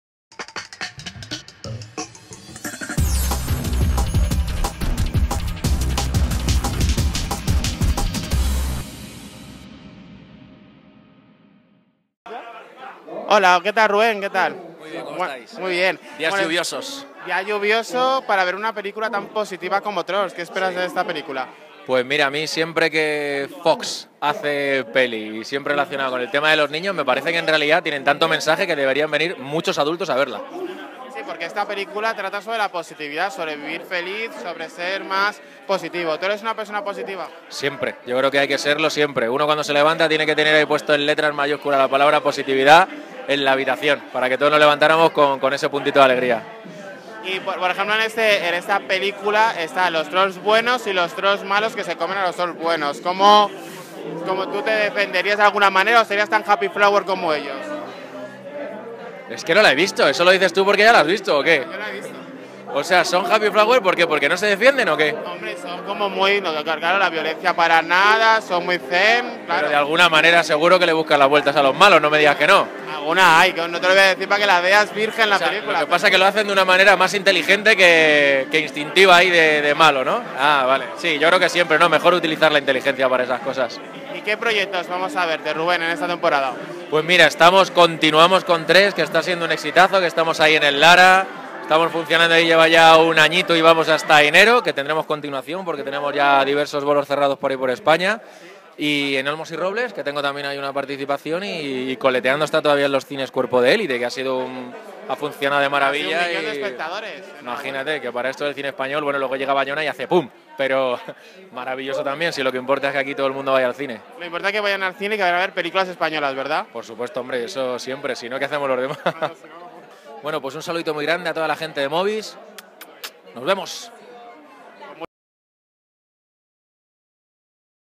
Trolls | Ruben Sanz
Hablamos con el actor Ruben Sanz en la premiere de Trolls